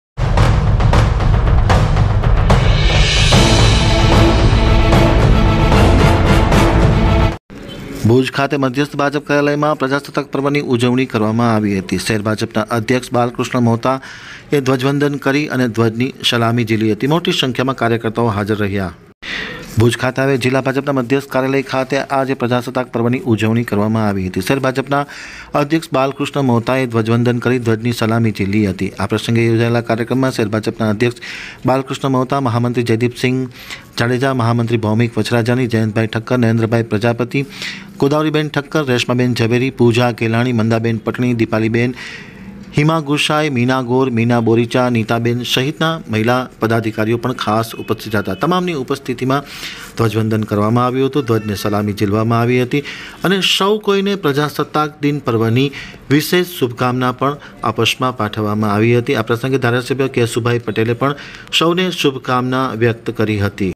AAMTAK_NEWS ભુજ ખાતે જિલ્લા મધ્યસ્થ ભાજપ કાર્યાલય માં શહેર ભાજપ દ્વારા ધ્વજવંદન
જિલ્લા મધ્યસ્થ ભાજપ કાર્યાલય ખાતે પ્રજાસત્તાક પર્વની ઉજવણી કરવામાં આવી હતી આ પ્રસંગે શહેર ભાજપના પ્રમુખ બાલકૃષ્ણ ભાઈ મોતા મહામંત્રી ભૌમિક વછરાજાની જયદીપસિંહ જાડેજા જયંત ઠક્કર નરેન્દ્ર પ્રજાપતિ ગોદાવરીબેન ઠક્કર રેશમાબેન ઝવેરી પૂજા ઘેલાણી, મંદાબેન પટણી દિપાલીબેન હીમાબેન મીનાબેન બોરીચા મીનાગોર નીતાબેન વગેરે હાજર રહી ભજનની સલામી જીલી હતી વિજય ઘેલાણી આમ તક ન્યુઝ ભુજ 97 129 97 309